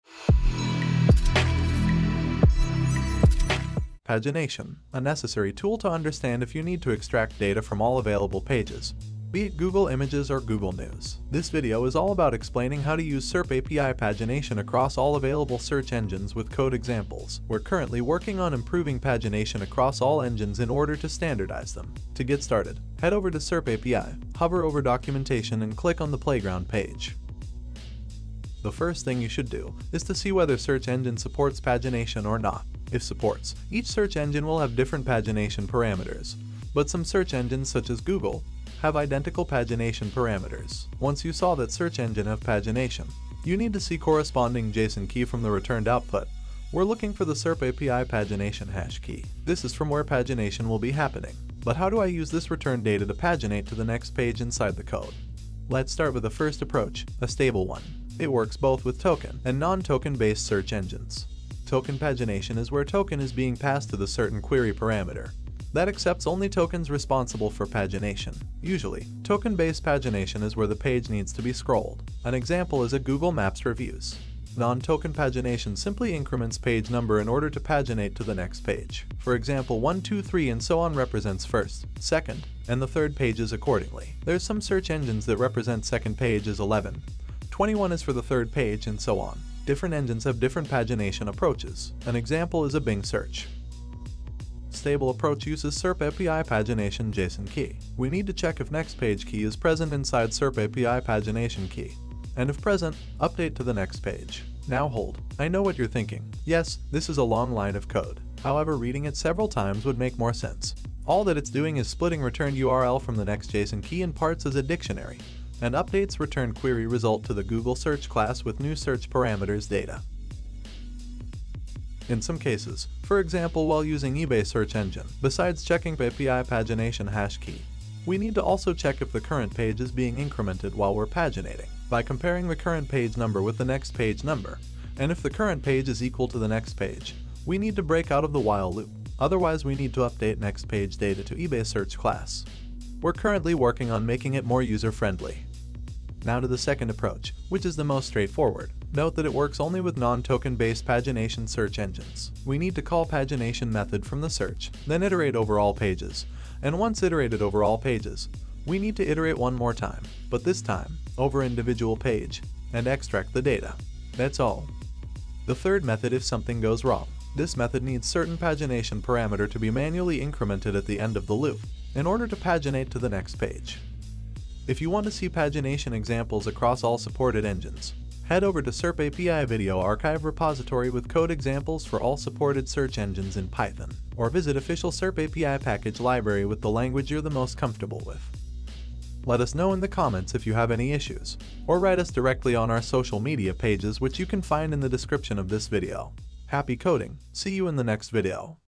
How to use SerpApi pagination across all APIs | Python examples
In this video, we'll go through SerpApi pagination, and how to use it across all available search engines. What is token and non-token-based pagination.

Please note that the following approaches of pagination are subject of change, they'll be fixed in an easier to use approach.

Chapters:
0:00 - Intro
0:04 - In this video...
0:21 - Getting started
1:00 - Token pagination
1:14 - Non-token pagination
1:34 - First method: works on all APIs
2:26 - Second method: works with non-token pagination
2:46 - Third method: manually passing pagination data
2:54 - Python pagination examples for all APIs
3:09 - Outro